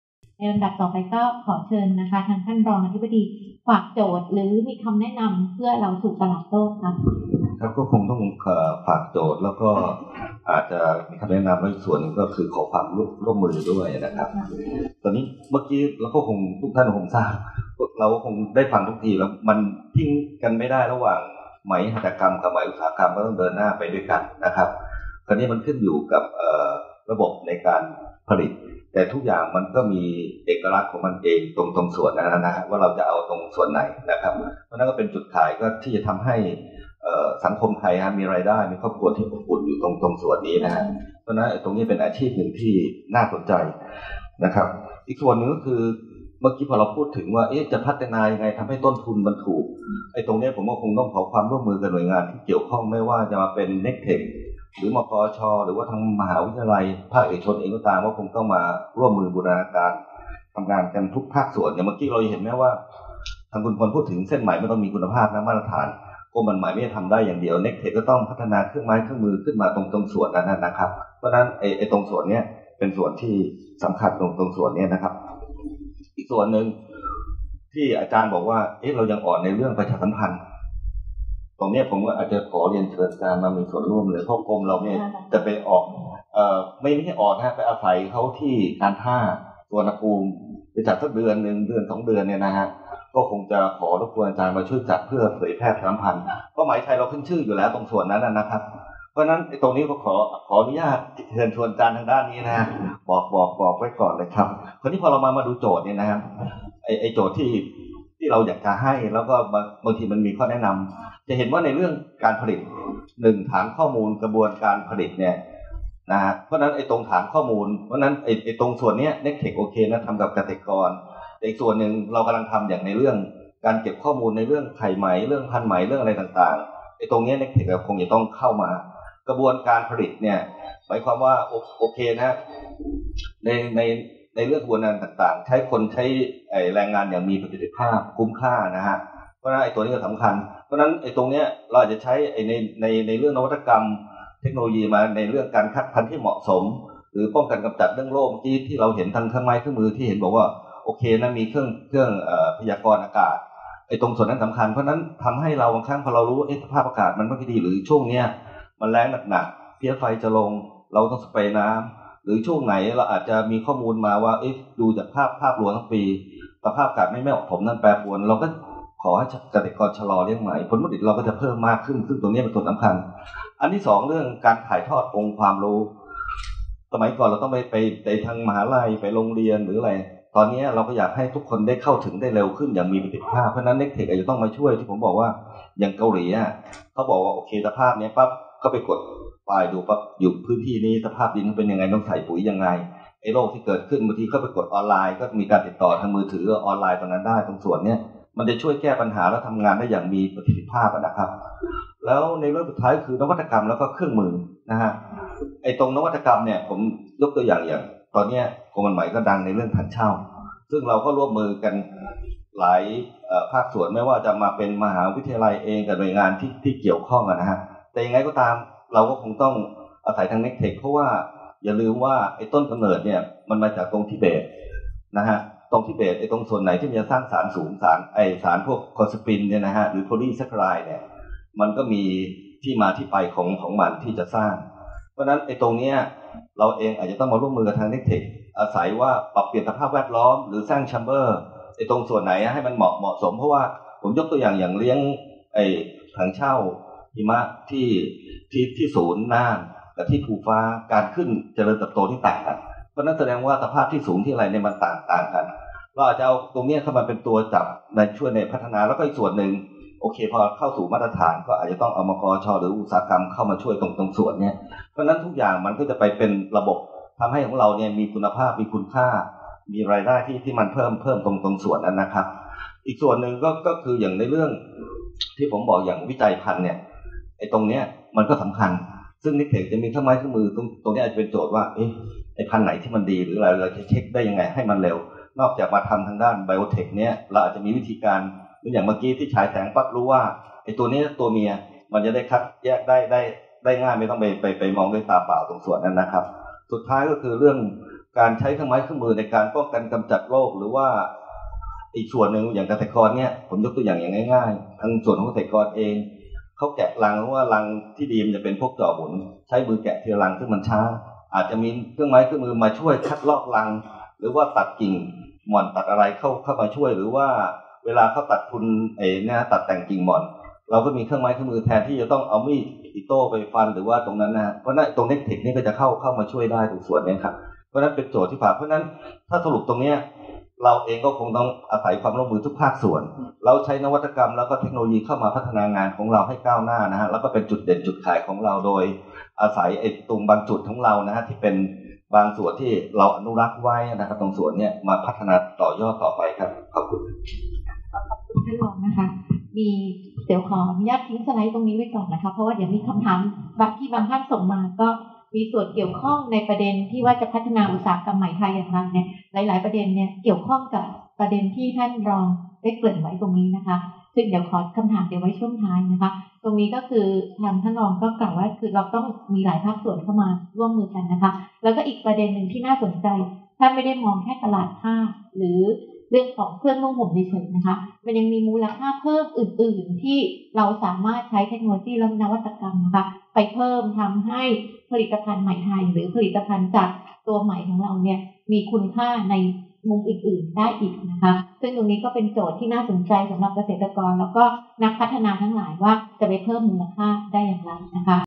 ยกระดับเกษตรกรรายย่อยไหมไทย สู่ตลาดโลกด้วยงานวิจัยอย่างครบวงจร (11/13)
การเสวนาเรื่อง ยกระดับเกษตรกรรายย่อยไหมไทย สู่ตลาดโลกด้วยงานวิจัยอย่างครบวงจร วันพุธที่ 1 เมษายน 2558 เวลา 9.00 – 12.00 น. ห้องประชุม NT-106 อาคารเนคเทค (NT) อุทยานวิทยาศาสตร์ประเทศไทย

การประชุมวิชาการประจำปี ๒๕๕๘ สำนักงานพัฒนาวิทยาศาสตร์และเทคโนโลยีแห่งชาติ “วิทยาศาสตร์ เทคโนโลยี และนวัตกรรม สู่ปวงไทย ด้วยน้ำพระทัยองค์สิรินธร” (Science, Technology, and Innovation: Princess Sirindhorn's Initatives for the benefit of Thai Society)